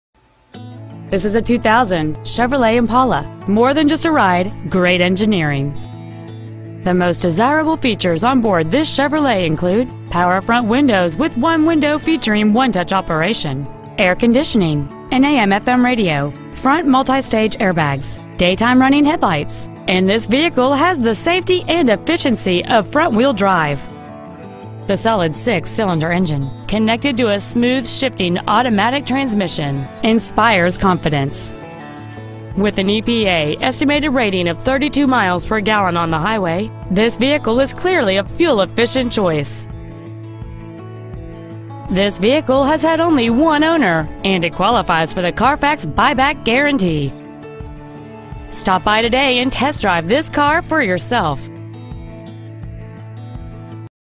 Used 2000 Chevrolet Impala Tulsa OK 74145
Riverside Toyota
 6868 East B.A. Frontage Rd.

 Year: 2000
 Make: Chevrolet
 Model: Impala
 Engine: 3.8L V-6cyl
 Trans.: 4-Speed Automatic
 Exterior: Green
 Miles: 1,388,684
 Interior: Tan